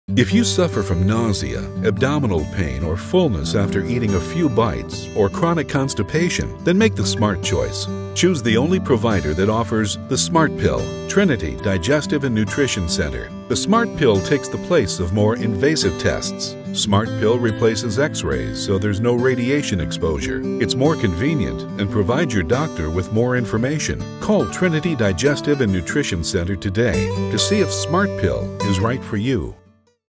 The Smart Pill
SmartPill is an ingestible capsule that measures pressure, pH and temperature as it travels through the gastrointestinal (GI) tract to assess GI motility.  The SmartPill motility monitoring test can be performed at a clinic or physician's office to evaluate motility disorders like gastroparesis (a condition in which the contents of the stomach empty too slowly) and chronic constipation.